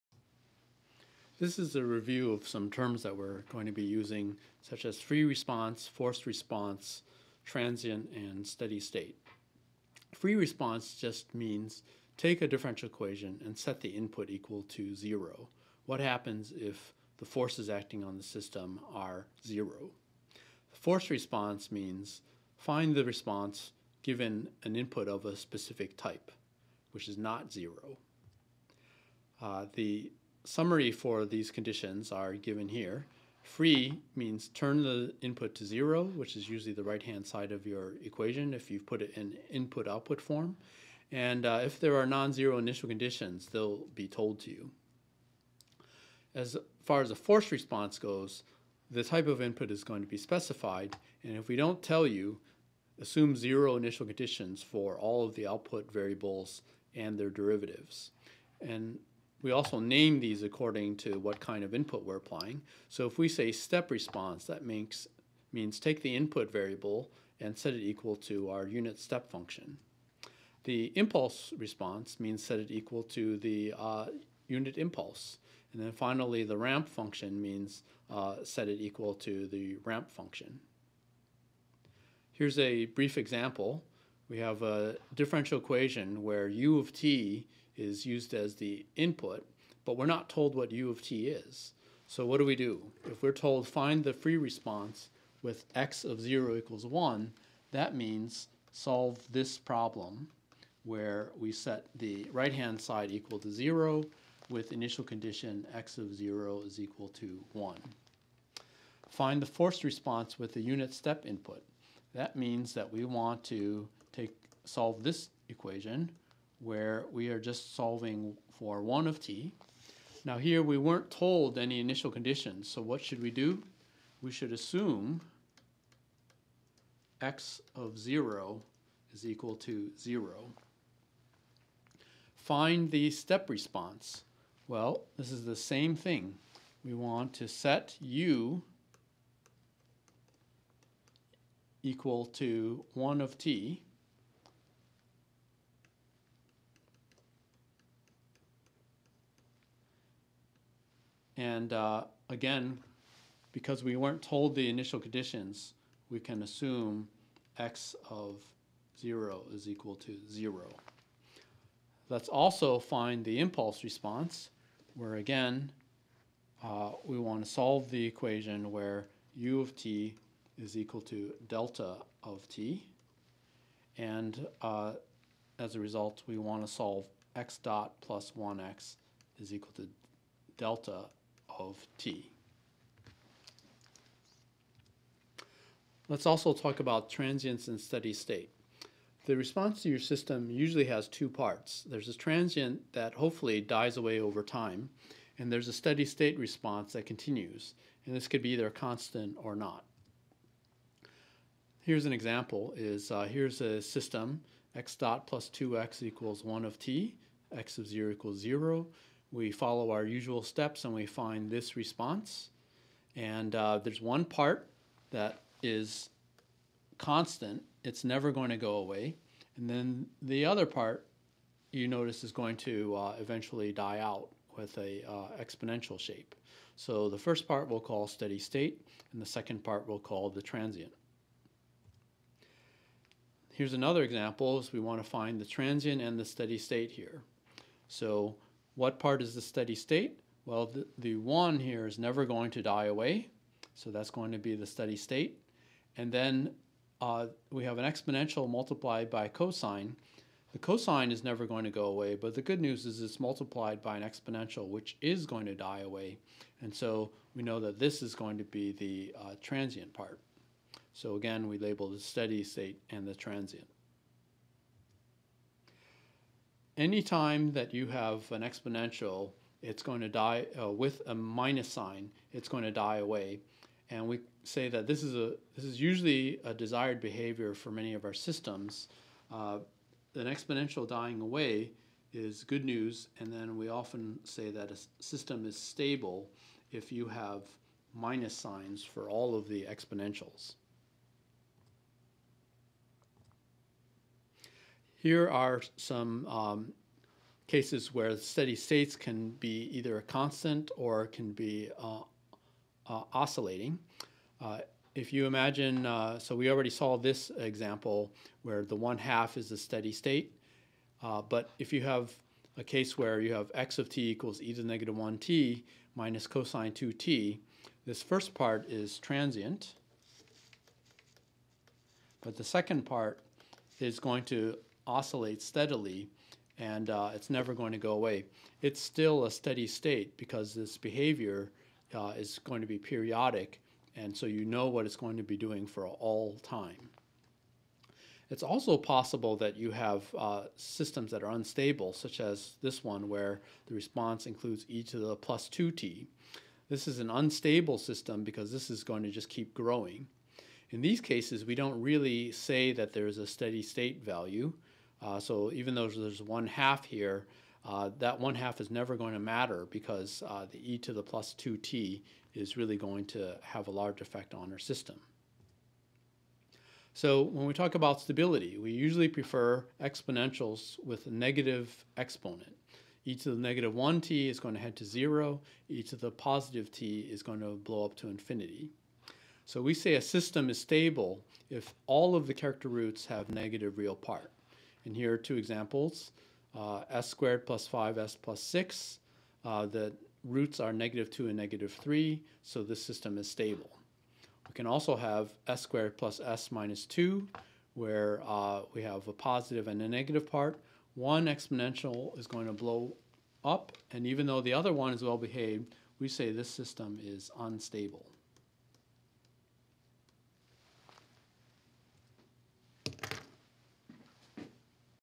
Basics: Terminology Forced and free responses, Transient and Steady state
Terms used for system response: Forced and free responses, transient and steady state portions of a response Supplementary video lectures for "Modeling, Analysis, and Control of Dynamic Systems," ME 360 Winter 2015, at University of Michigan.